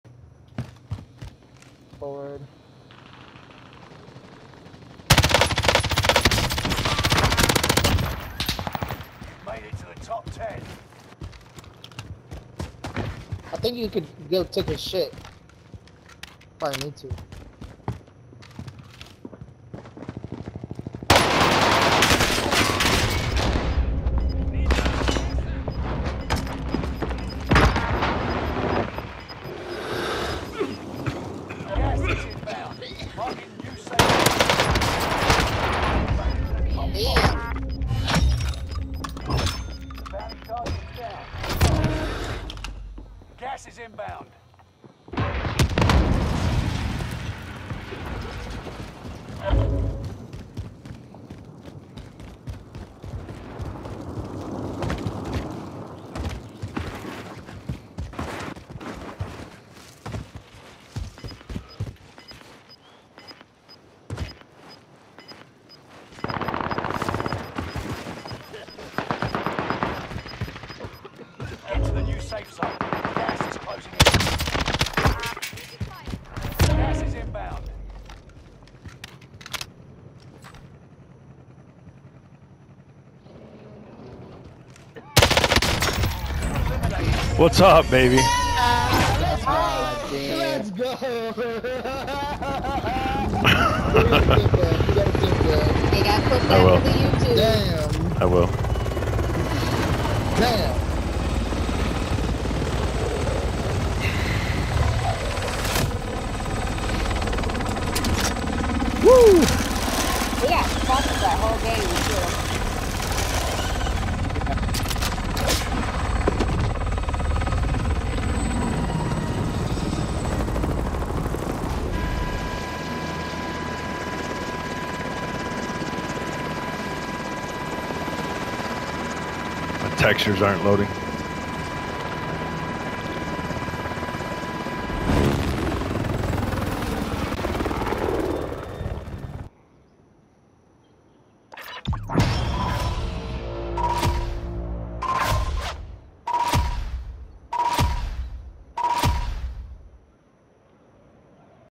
Clutched the dub with the M13 and MP40
The odds were against me but good positioning helps